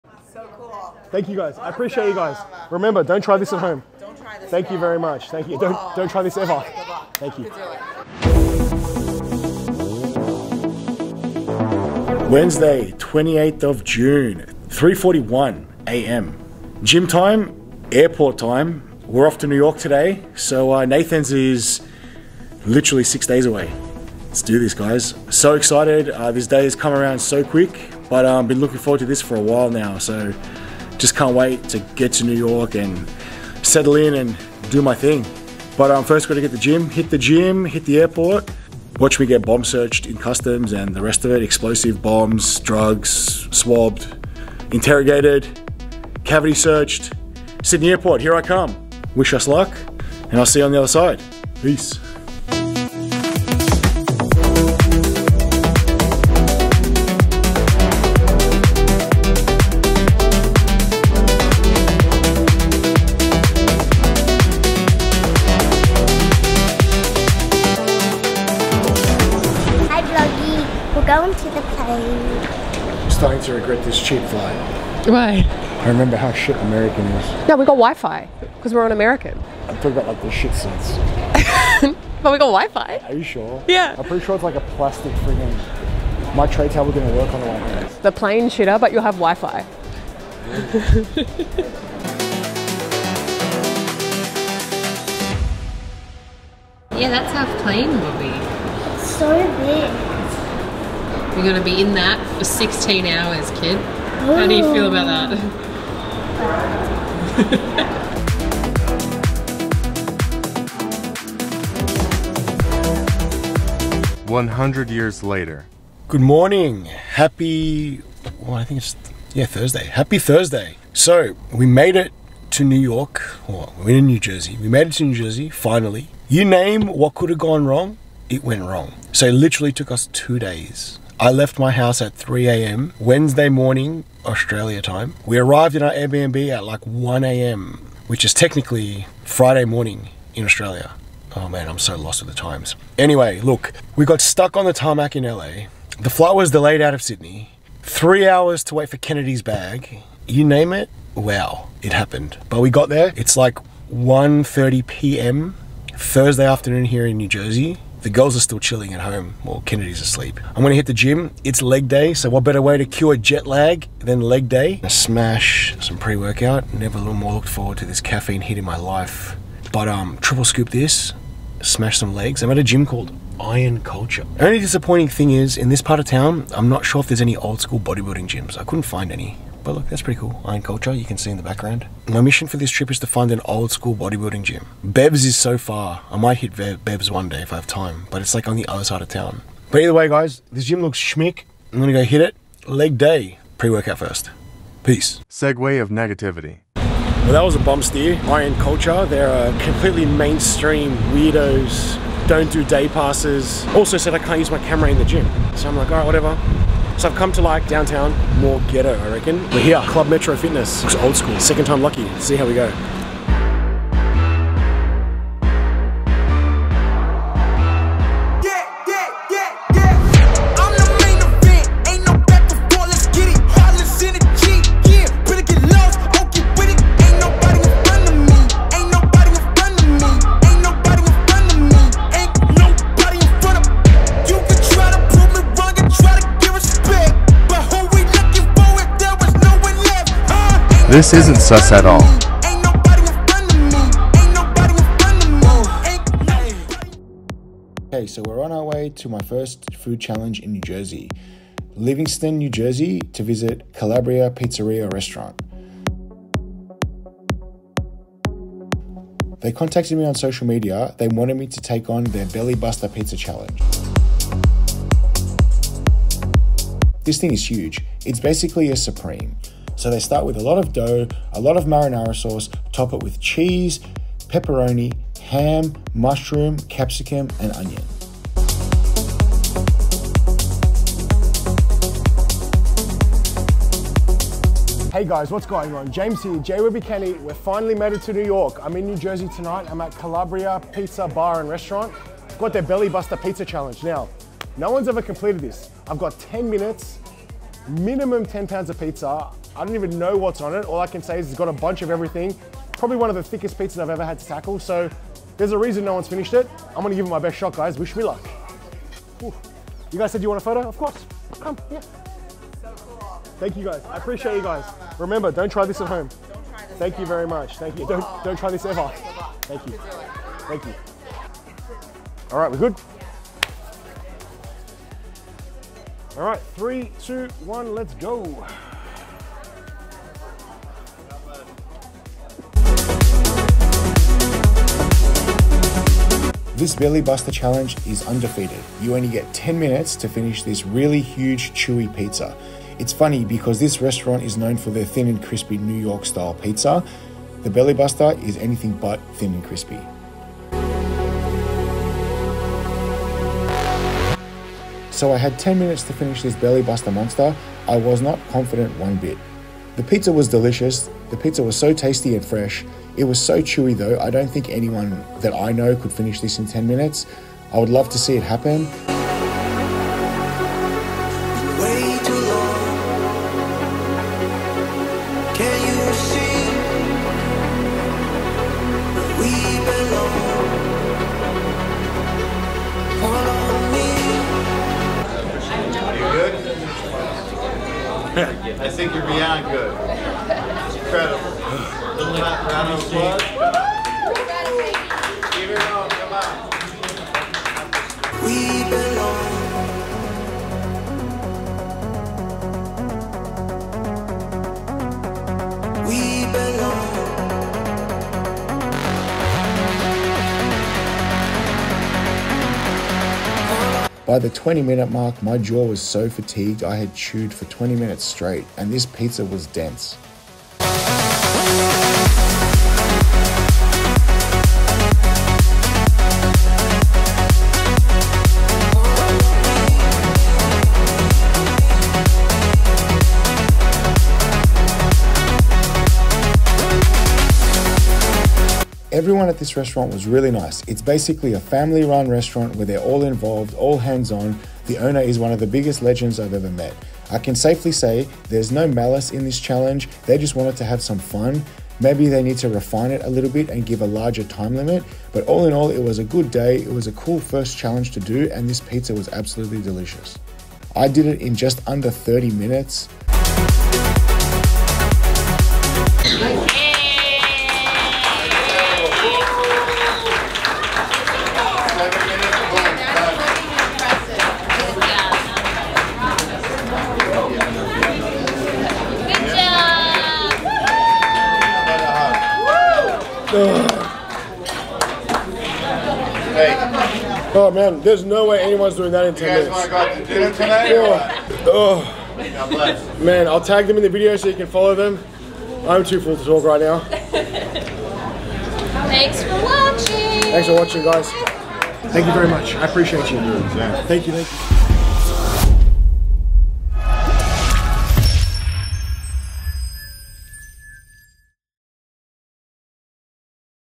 10LBS IN 10MINS????? BELLY BUSTER CHALLENGE - CALABRIA PIZZERIA & RESTAURANT
James Webb aka J WEBBY CAN EAT attempts Calabria Pizzeria & Restaurant 'BELLY BUSTER CHALLENGE' in Livingston, New Jersey

Calabria Pizzeria & Restaurant
588 S Livingston Ave
Livingston, NJ 07039, USA

✘ I N S T A G R A M ∼ jwebby_can.eat
✘ T I K T O K ∼ jwebby_can.eat
✘ T W I T C H ∼ jwebbycaneat